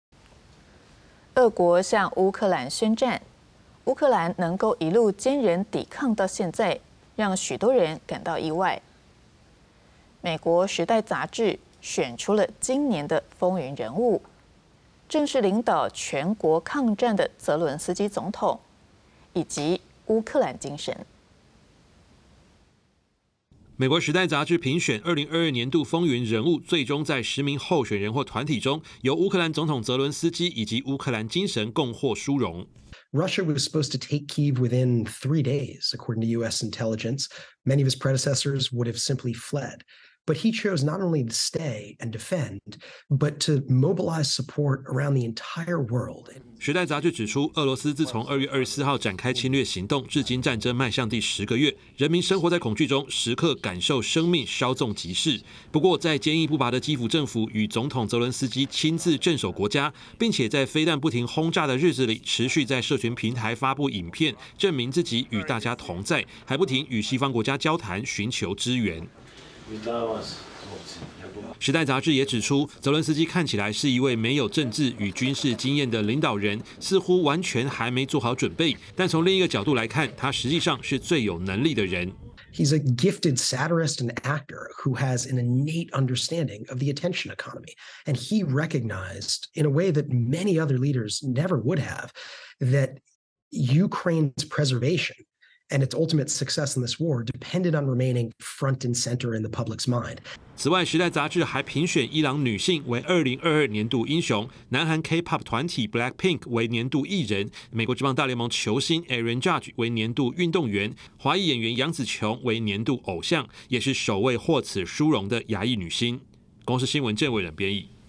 時代風雲人物 澤倫斯基.烏克蘭精神獲選 | 20221209 公視手語新聞
俄國向烏克蘭宣戰，烏克蘭能夠一路堅忍抵抗到現在、讓許多人感到意外。美國"時代雜誌"選出今年的風雲人物、正是領導全國抗戰的"澤倫斯基"總統，以及"烏克蘭精神"。

🙌每週一到週五 早上8:00-8:30 歡迎收看